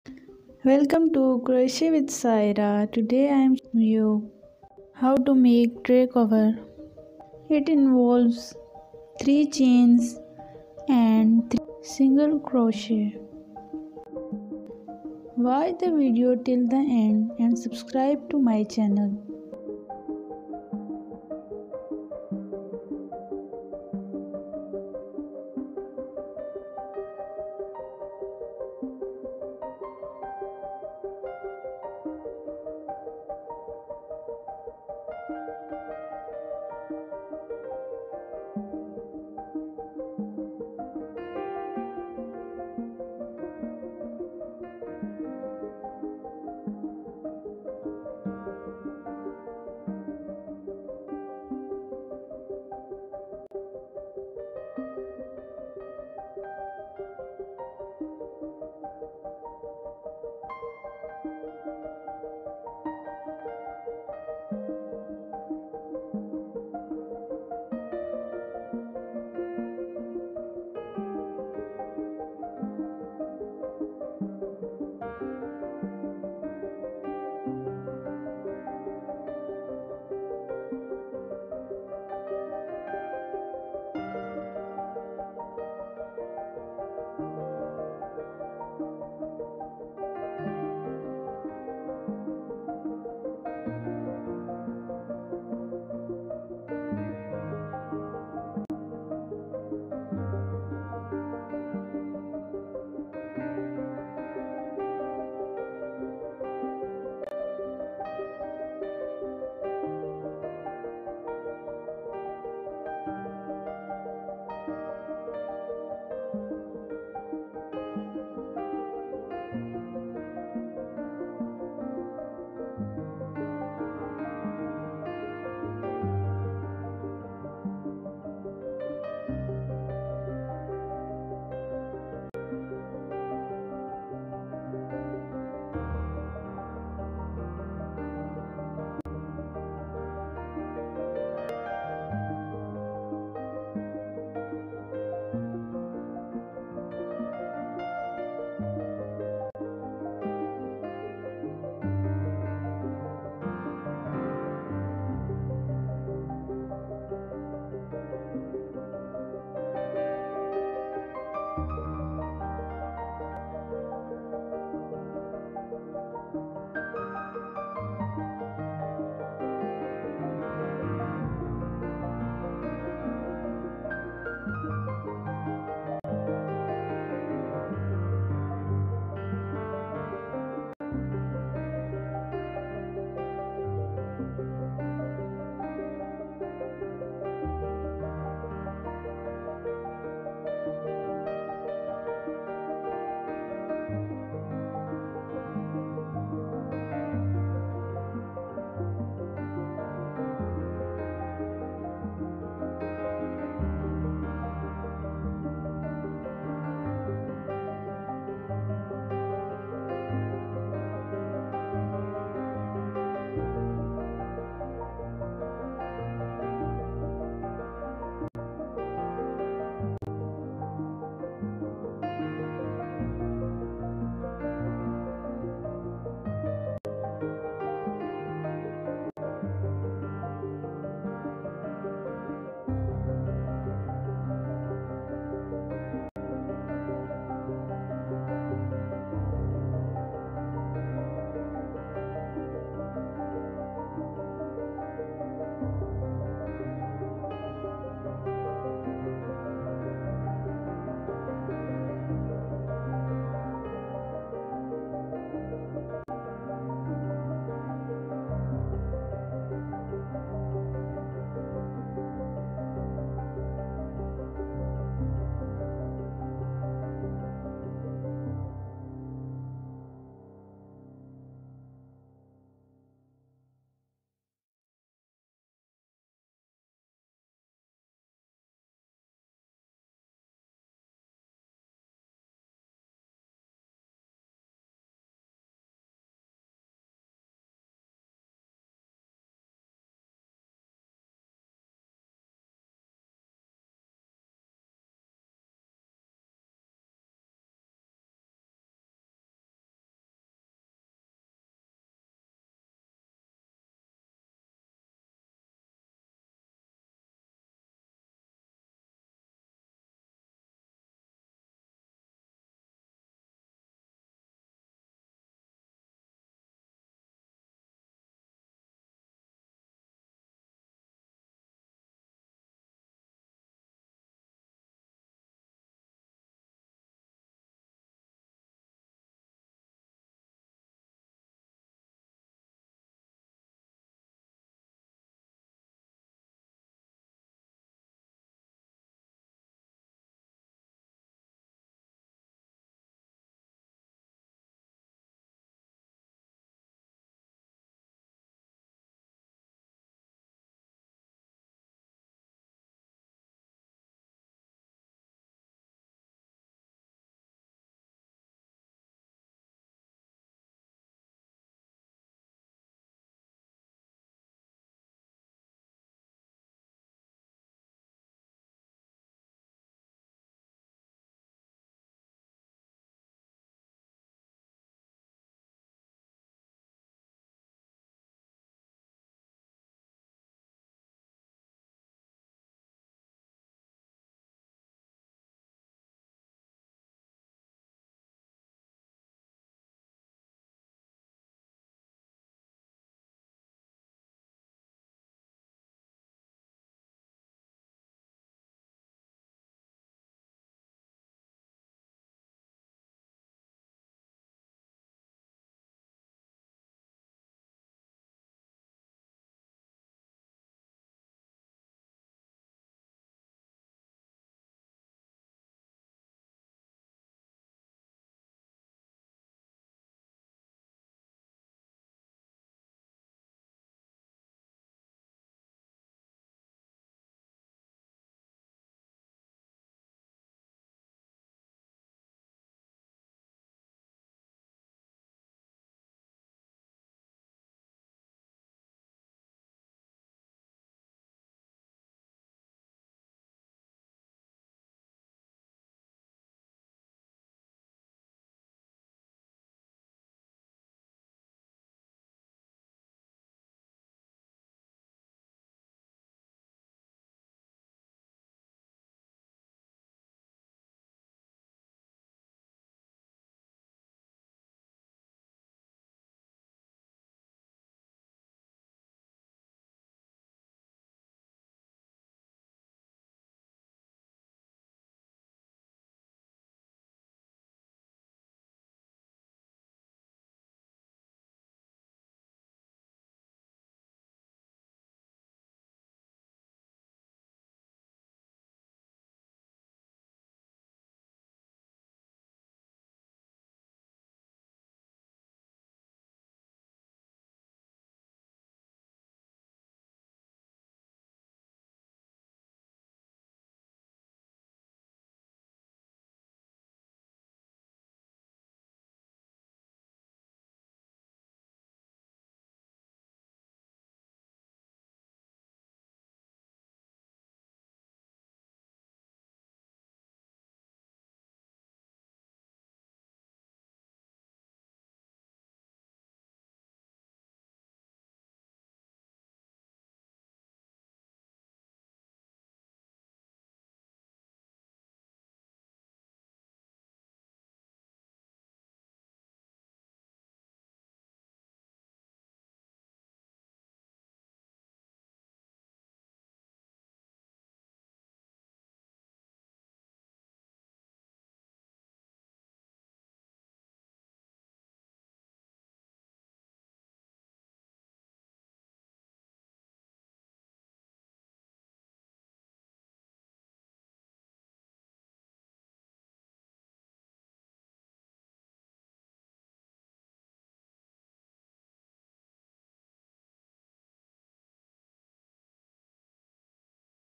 Crochet tray cover easy for beginners| Amazing😍 tray cover
Welcome 😊 to Crochet with Saira

To make tray cover you just need to know basic two stitches chain and single crochet

Procedure
First make chain of length equal to tray cover and then make 3 chain and 3 Single crochet alternative along the length of simple chain.
Make 3 rows with each color or yarn in my case I used yellow and white. Then make border with white yarn .

This is simple and easy way to make tray Cover even beginners can do this.

crochet for beginners
crochet for absolute beginners
crochet my hobby
crochet tray cover